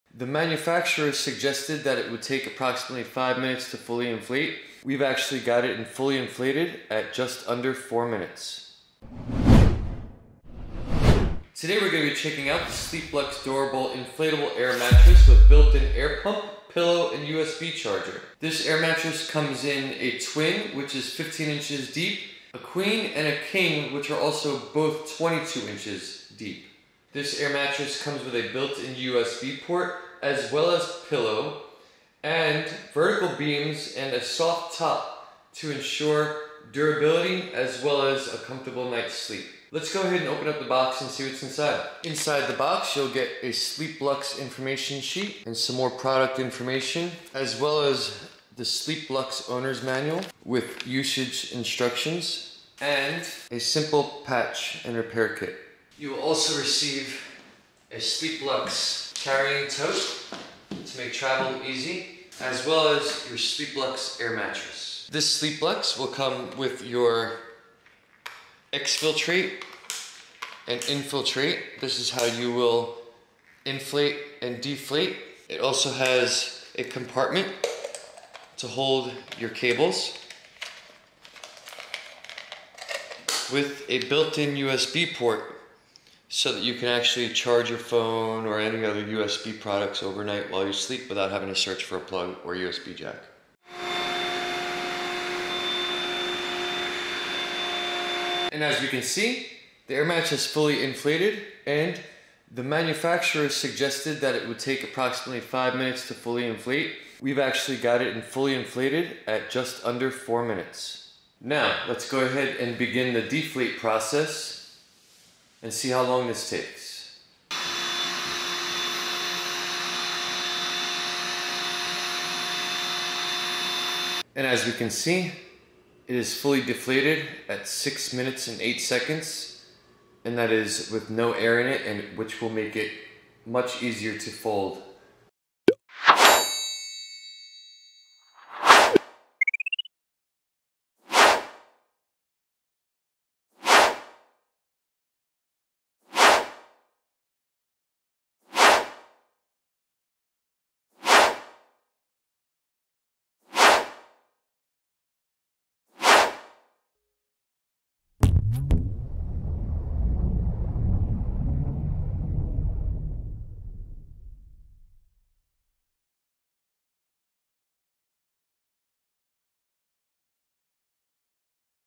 SLEEPLUX Durable Inflatable Air Mattress Review
SLEEPLUX Durable Inflatable Air Mattress
Amazon INTL

SLEEPLUX Durable Inflatable Air Mattress with Built-in Pump, Pillow and USB Charger

00:00 - Intro
00:41 - Contents
01:05 - Controls
01:33 - Inflating
01:38 - Inflated
01:51 - Deflate